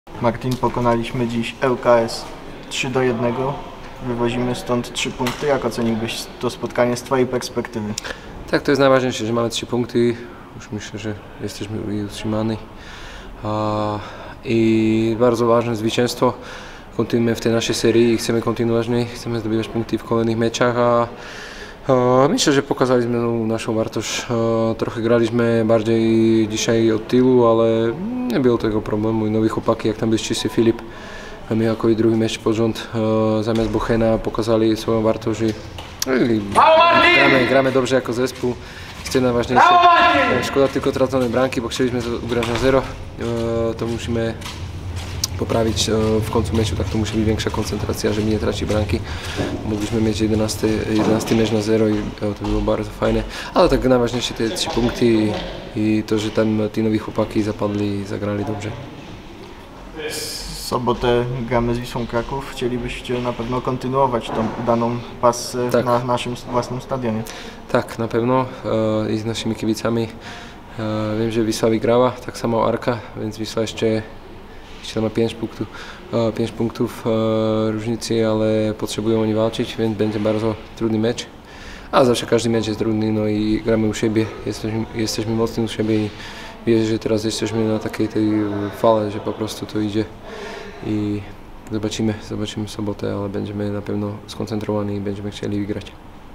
Martin Chudy o meczu z ŁKS
Pomeczowy komentarz bramkarza zabrzańskiego Górnika, Martina Chudego.